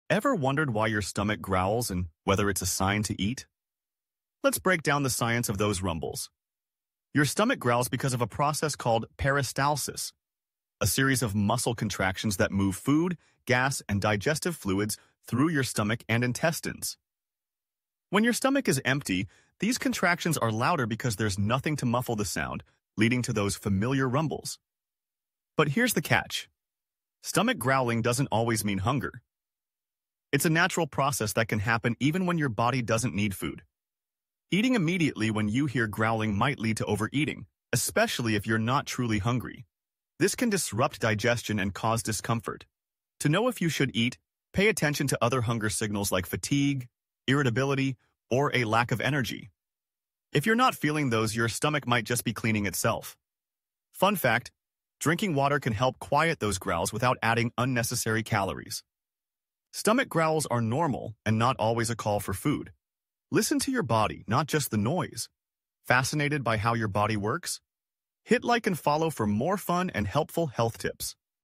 Why Does Your Stomach Growl and Should You Eat When It Does?
"Why does your stomach growl, and does it mean you should eat? Learn the science behind stomach rumbles and how to listen to your body properly."